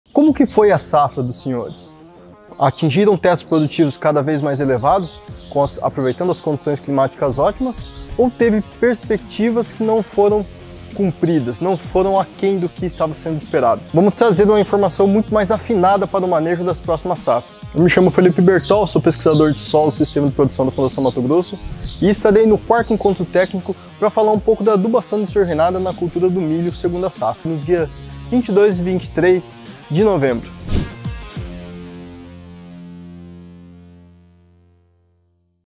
IV Encontro Técnico Milho: Informações e resultados da adubação Nitrogenada na cultura do milho.
A Adubação Nitrogenada está entre os temas que serão abordados no IV Encontro Técnico Milho, da Fundação MT. O pesquisador, Felipe Bertol, estará compartilhando percepções e resultados afinados obtidos na 2ª Safra de 2023 🌽📊.

Uma oportunidade ímpar para quem busca alavancar os resultados na produção de milho. Participe conosco, acesse o link na bio!

📅 22 e 23 de novembro de 2023

📍 Hotel Gran Odara, Cuiabá MT